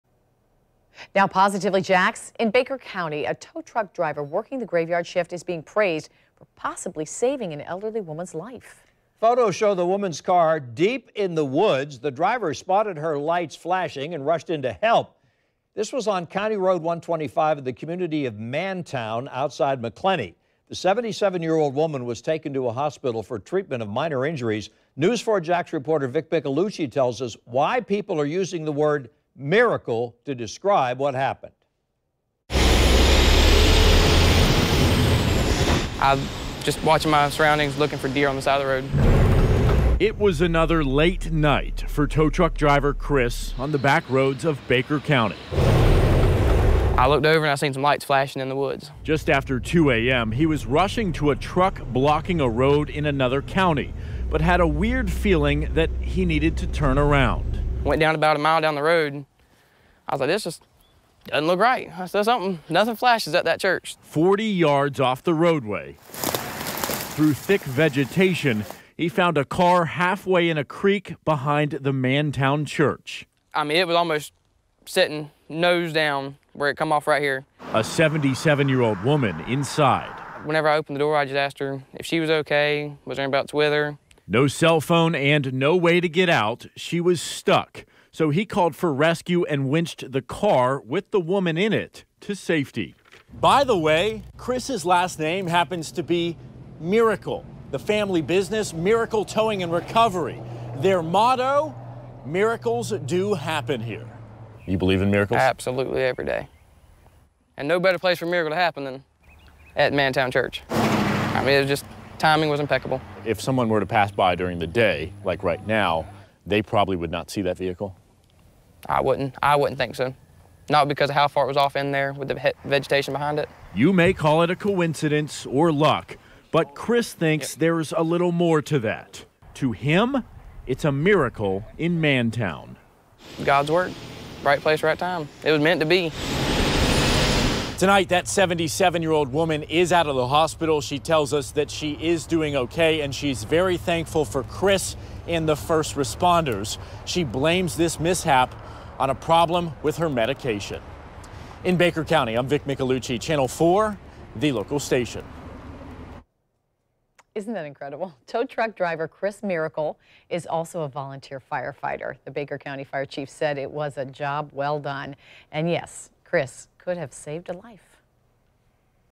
Tow truck driver rescues woman
A tow truck driver working the graveyard shift is being praised for possibly saving an elderly woman's life.